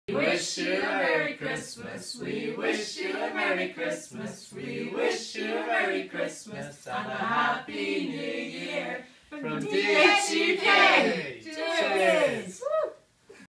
Merry Christmas
A Christmas message to all the DSUK followers from HQ!